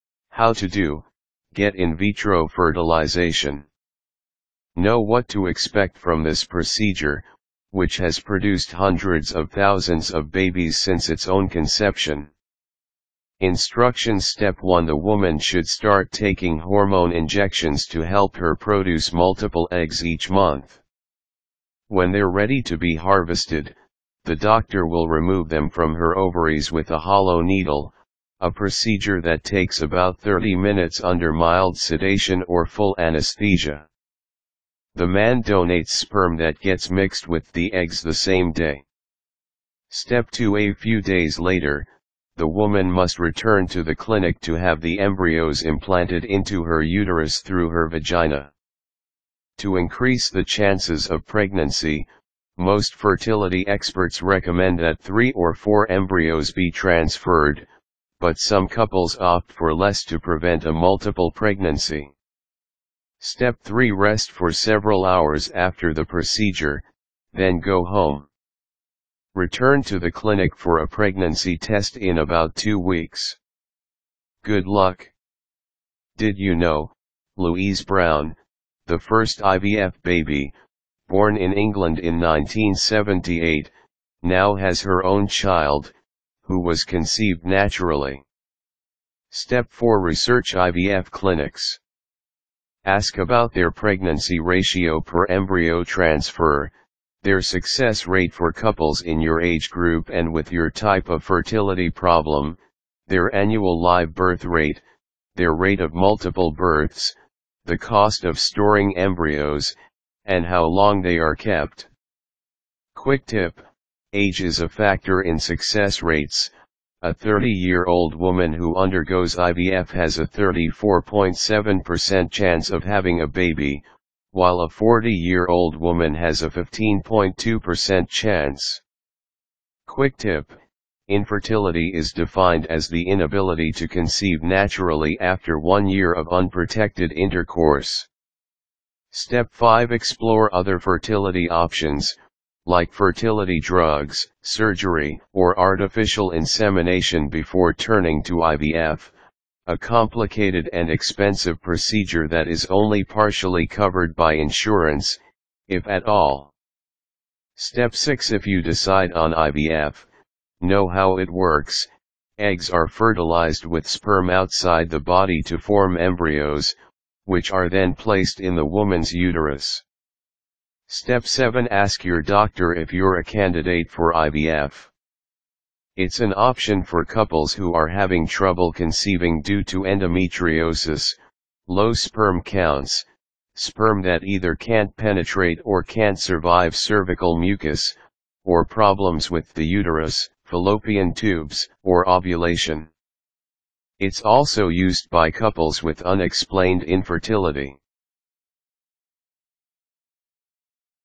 How to do: Get in Vitro Fertilization
Know what to expect from this procedure, which has produced hundreds of thousands of babies since its own conception.

 Instructions  Step 1 The woman should start taking hormone injections to help her produce multiple eggs each month. When they're ready to be harvested, the doctor will remove them from her ovaries with a hollow needle, a procedure that takes about 30 minutes under mild sedation or full anesthesia. The man donates sperm that gets mixed with the eggs the same day.

 Step 2 A few days later, the woman must return to the clinic to have the embryos implanted into her uterus through her vagina. To increase the chances of pregnancy, most fertility experts recommend that three or four embryos be transferred, but some couples opt for less to prevent a multiple pregnancy.

 Step 3 Rest for several hours after the procedure; then go home. Return to the clinic for a pregnancy test in about two weeks. Good luck!  Did You Know:

 Louise Brown, the first IVF baby, born in England in 1978, now has her own child, who was conceived naturally. Step 4 Research IVF clinics. Ask about their pregnancy ratio per embryo transfer, their success rate for couples in your age group and with your type of fertility problem, their annual live birth rate, their rate of multiple births, the cost of storing embryos, and how long they are kept.  Quick Tip:

 Age is a factor in success rates: A 30-year-old woman who undergoes IVF has a 34.7 percent chance of having a baby, while a 40-year-old woman has a 15.2 percent chance.  Quick Tip:

 Infertility is defined as the inability to conceive naturally after one year of unprotected intercourse. Step 5 Explore other fertility options – like fertility drugs, surgery, or artificial insemination –before turning to IVF, a complicated and expensive procedure that is only partially covered by insurance, if at all.

 Step 6 If you decide on IVF, know how it works: Eggs are fertilized with sperm outside the body to form embryos, which are then placed in the woman's uterus.

 Step 7 Ask your doctor if you're a candidate for IVF. It's an option for couples who are having trouble conceiving due to endometriosis; low sperm counts; sperm that either can't penetrate or can't survive cervical mucus; or problems with the uterus, fallopian tubes, or ovulation. It's also used by couples with unexplained infertility.